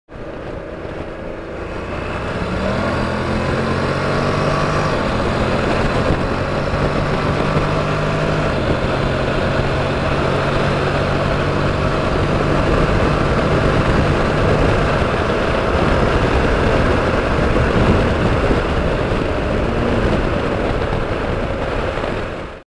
2016 Honda NC700XD Passing Five Cars on Santiago Canyon Road
It's important for motorcyclists to "ride their own ride," meaning to prioritize personal safety, and to not feel any pressure to follow the lead rider's actions.

In this video, riding buddy Peter, on his 2022 BMW R1250RS (136 hp and 105 lb-ft of torque) passes a slower-moving line of five cars on Santiago Canyon Road in Orange County, California, on Sunday, May 14, 2023.

Even though my 2016 Honda NC700XD is far less powerful (only 51 hp and 45 lb-ft of torque) I visually judged that I had enough time and space to safely pass all the cars.